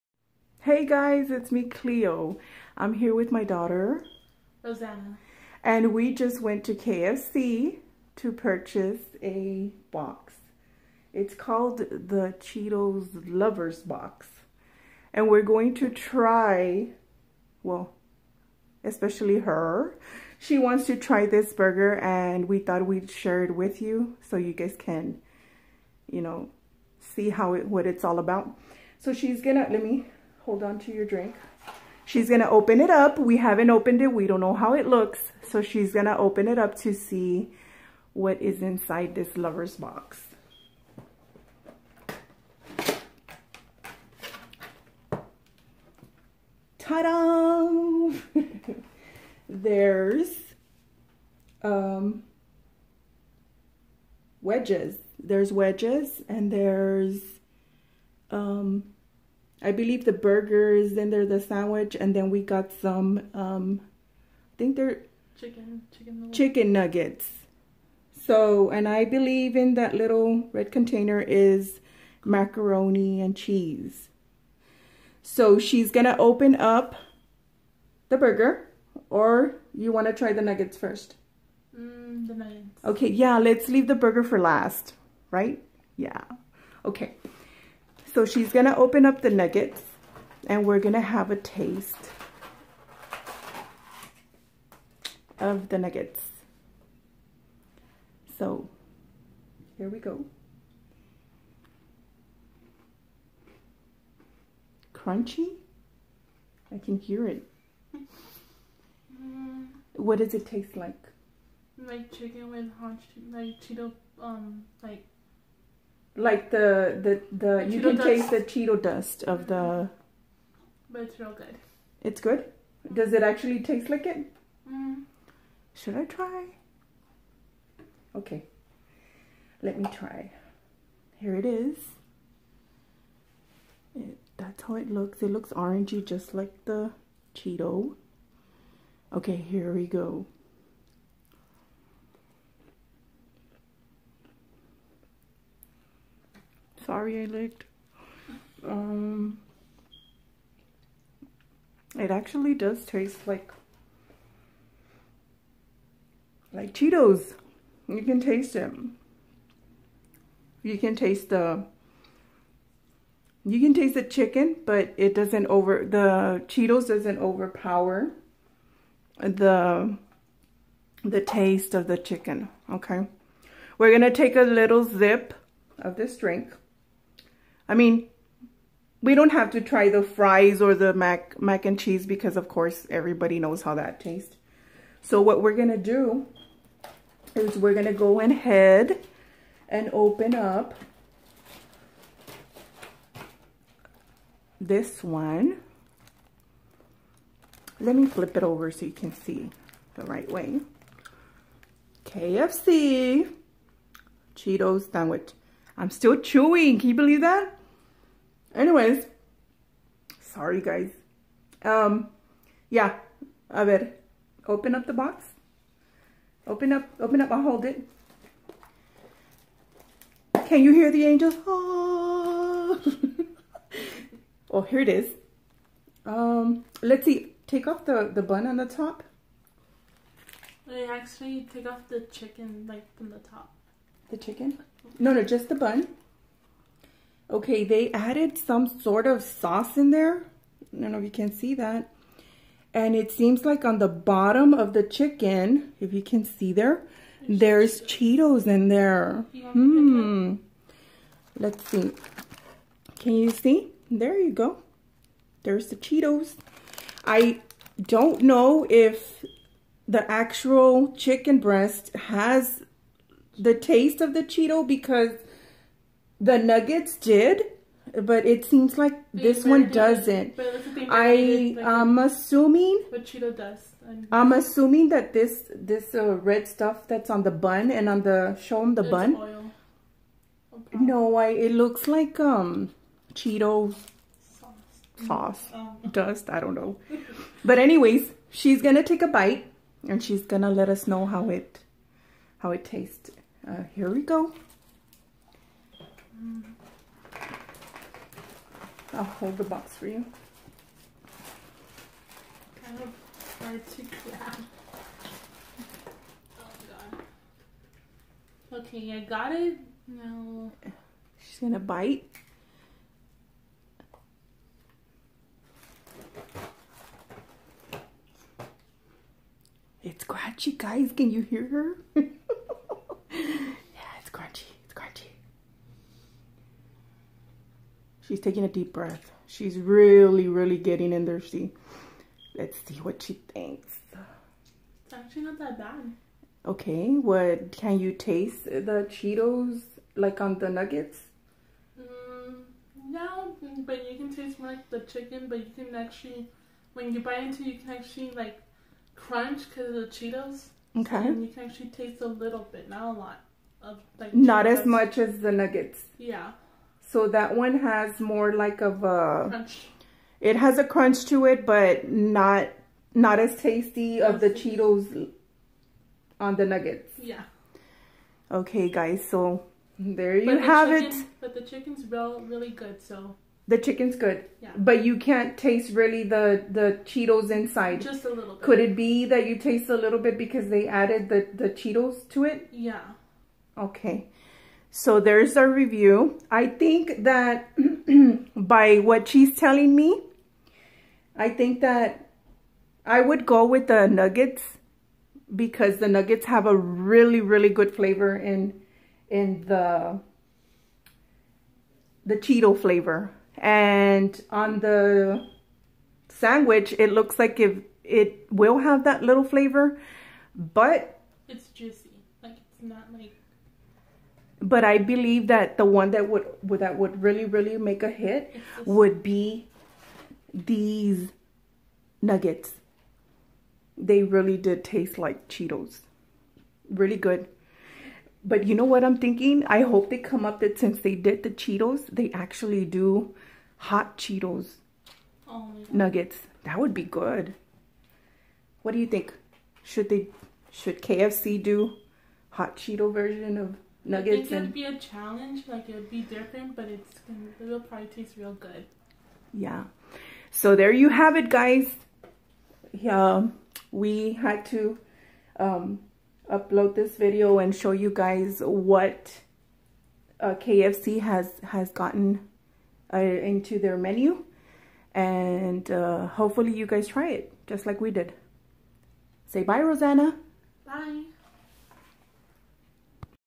New KFC Cheetos Chicken Sandwich Lovers Box
Lovers Box comes with sandwich and popcorn  nuggets,  drink potatoe wedges and Mac and cheese 😁😁😁